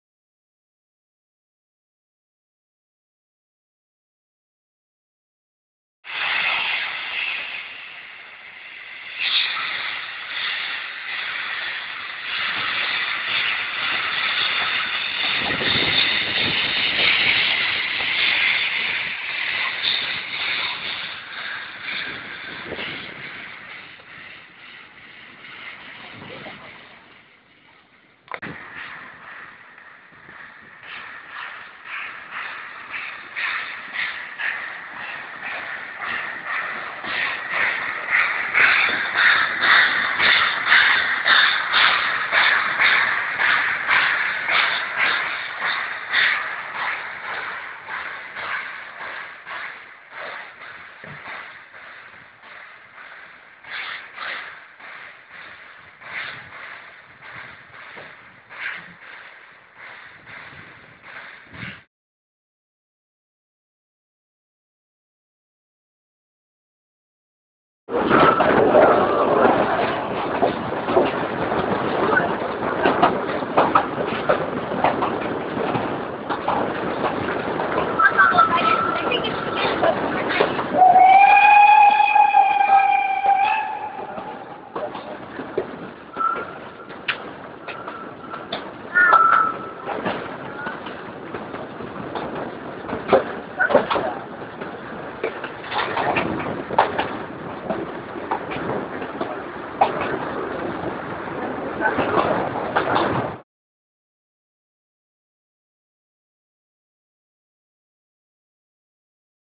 Rasender Roland, Rügen
Der Rasende Roland auf Rügen: Gefilmt beim Rangieren im Bahnhof und in voller Fahrt im Mai 2008!